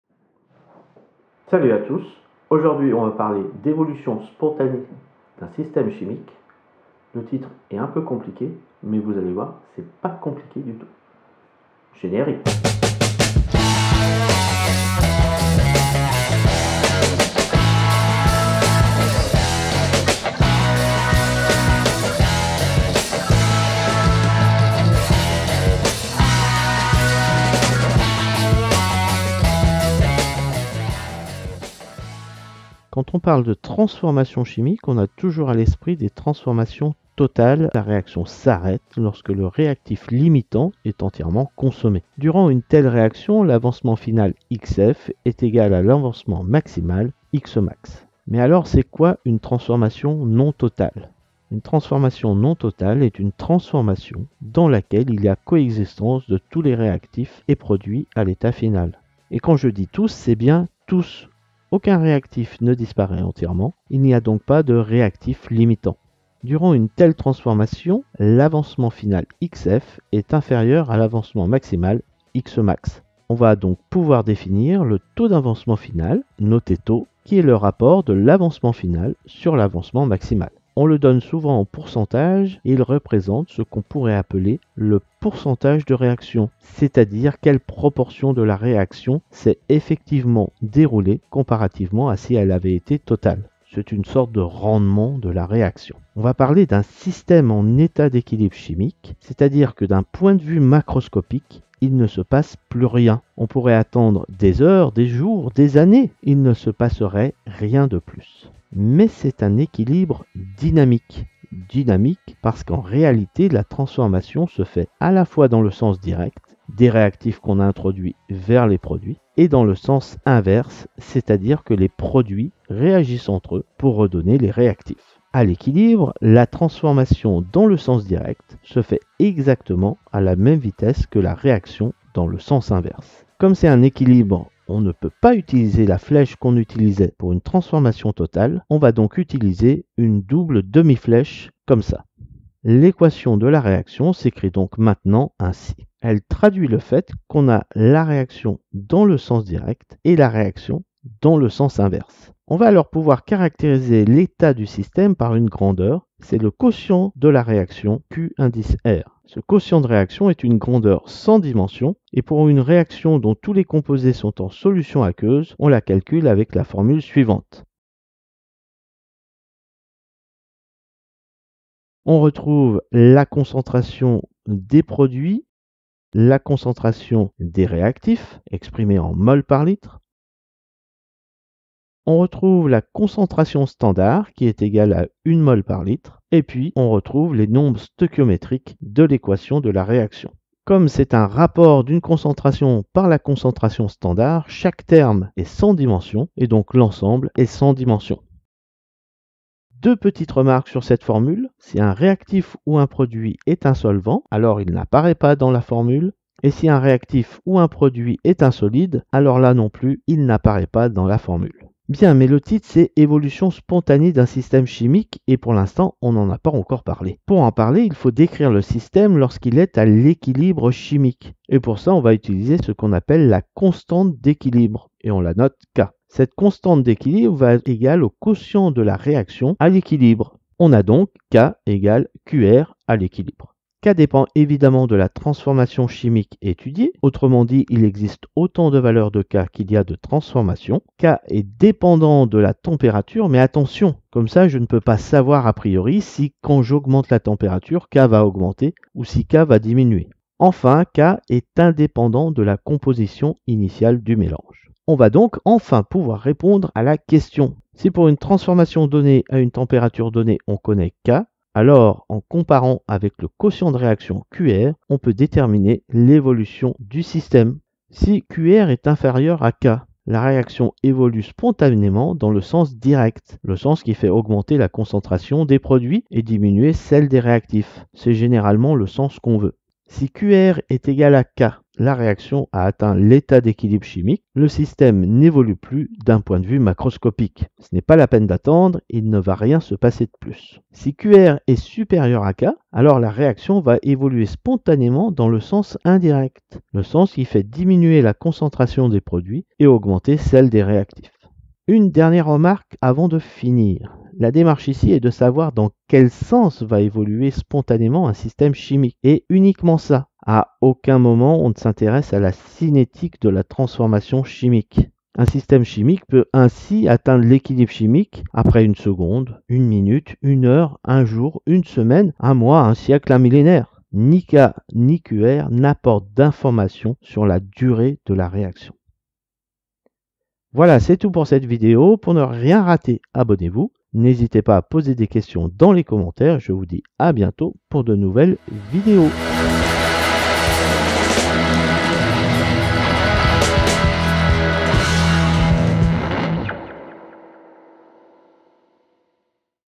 L'évolution spontanée d'un système chimique en moins de 5 minutes
Lorsqu'on mélange des réactifs, c'est à dire qu'on a un système chimique, il va y avoir transformation chimique.
La transformation sera-t-elle toujours totale ?
Comment caractériser le système chimique ?
Peut on prévoir l'évolution d'un système chimique ?  ?

En moins de 5 minutes, l'explication pour tout comprendre (enfin j'espère ! 😅)